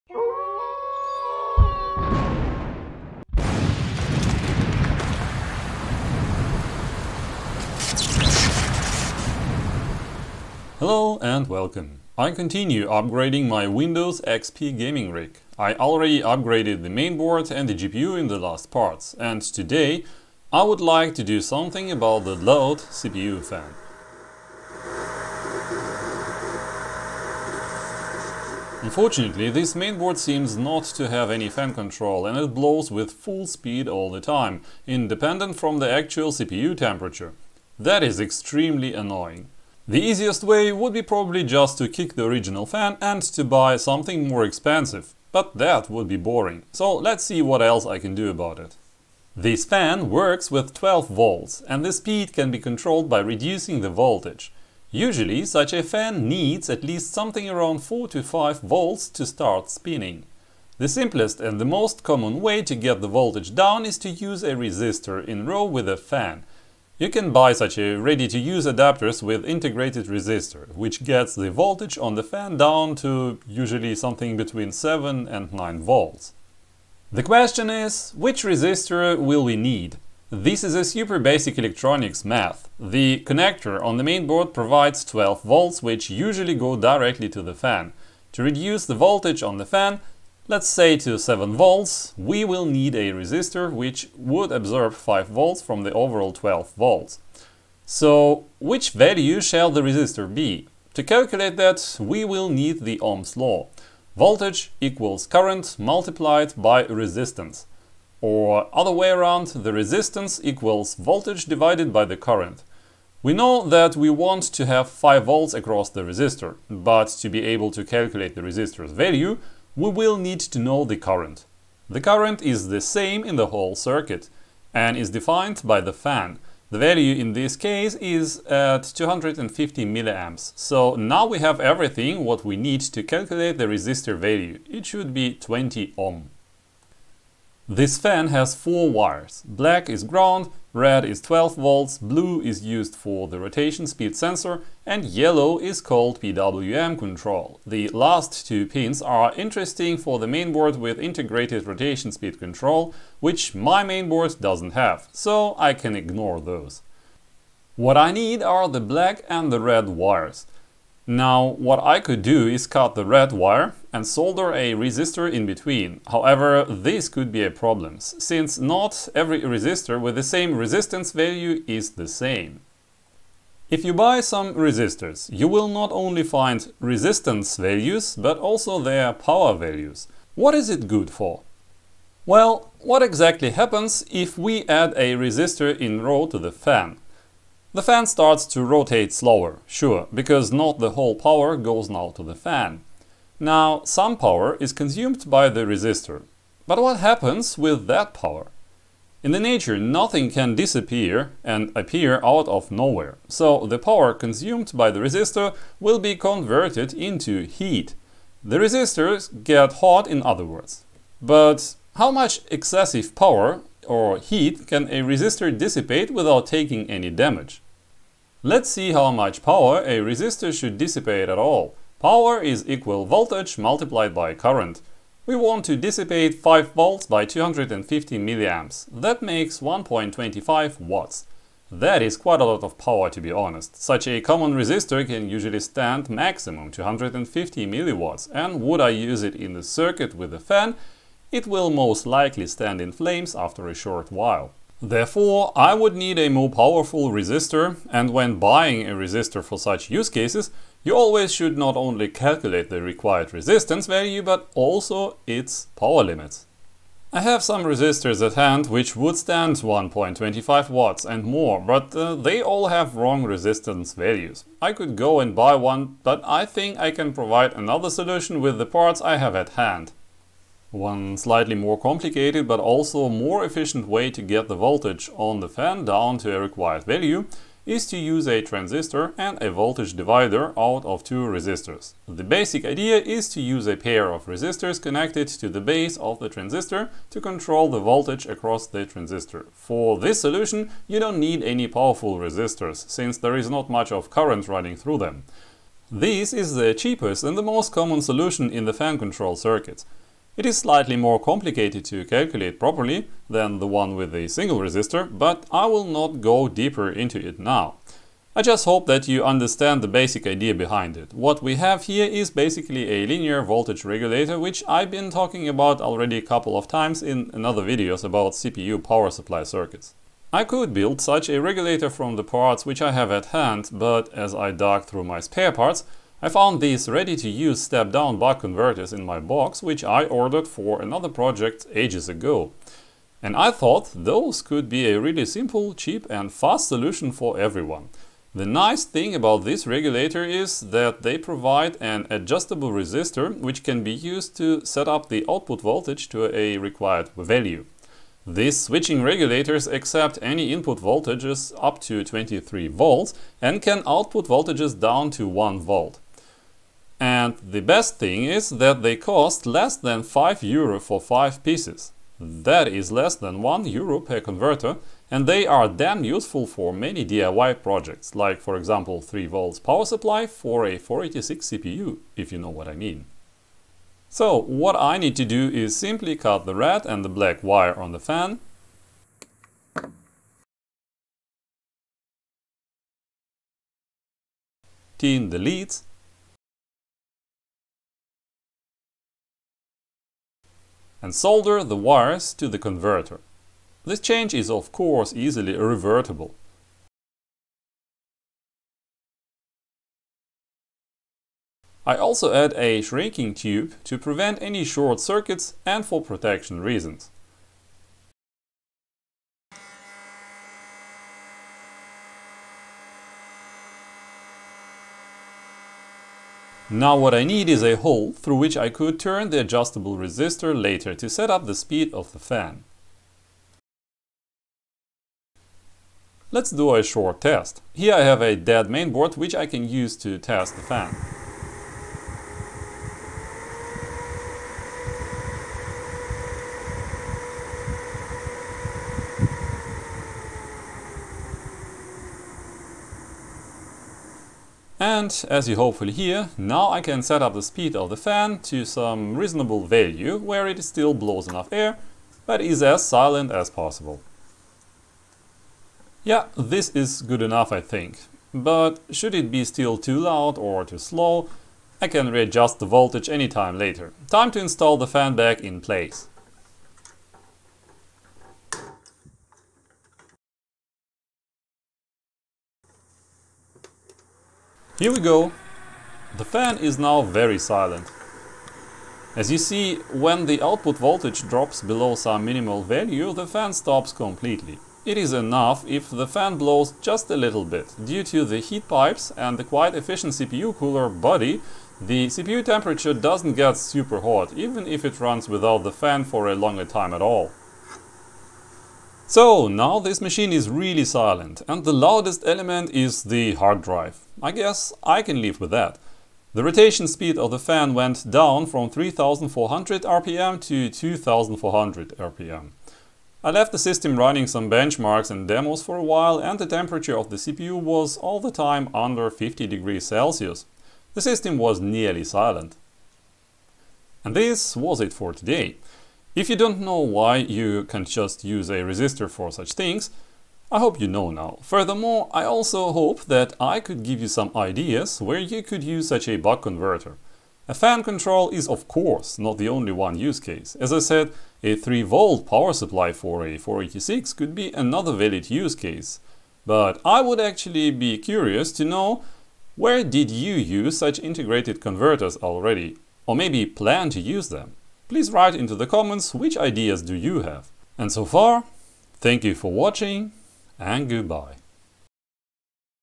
Noisy CPU fan? Here are some ideas.
In this video I countinue the work on my Windows XP machine and I would like to do something about the noisy CPU fan. May be you can find some information and ideas from this video useful for your own projects.